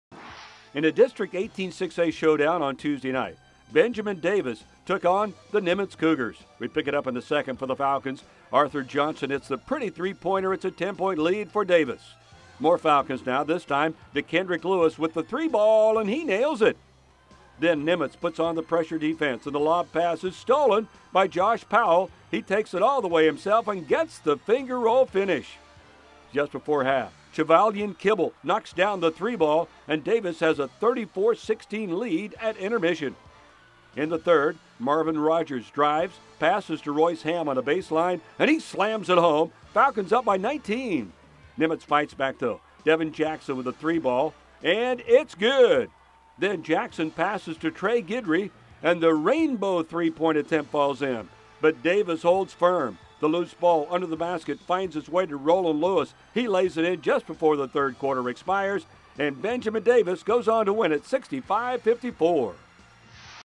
Aldine Davis Falcons vs. Nimitz Cougars - 2014/15 Boys Basketball - 01/27/15
BEN DAVIS vs NIMITZ – 1/27/15

1 – District 18-6A showdown on Tuesday night saw the Benjamin Davis Falcons go up against the Nimitz Cougars

2 – We pick it up in the second, for the Falcons, ARTHUR JOHNSON hits the pretty three pointer, 10 point lead for Davis

3 – More Falcons, this time it’s D’KENDRICK LEWIS with the three, and he nails it

4 – Then, Nimitz puts on the pressure defense, and the lob pass is picked off by JOSH POWELL, and he takes it all the way himself, and the finger roll finish

5 – Just before half, CHOVALLIAN KIBBLE knocks down the three pointer, and Davis had a 34-16 lead at the break

6 – Third quarter, MARVIN RODGERS drives, passes to ROYCE HAMM on the baseline and he slams it in. Falcons up by 19

7 – Nimitz fights back. DEVIN JACKSON with the three and it’s good

8 – Then, JACKSON passes to TREY GUIDRY, and the rainbow three point attempt falls in

9 – But Davis held firm, the loose ball under the basket finds ROLAND LEWIS, and he lays it in just before the third quarter expires.  Benjamin Davis wins 65-54